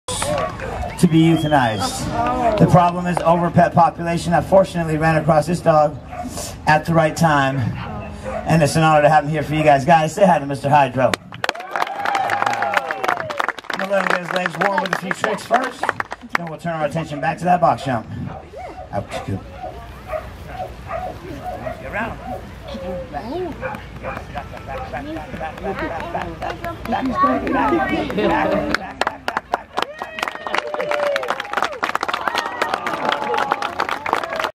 Canines in the Clouds 7-1-17 Video 8
Video 8- Canines in the Clouds dog show July 1, 2017. This show was amazing and the dogs seemed to love it just as much as the crowd did!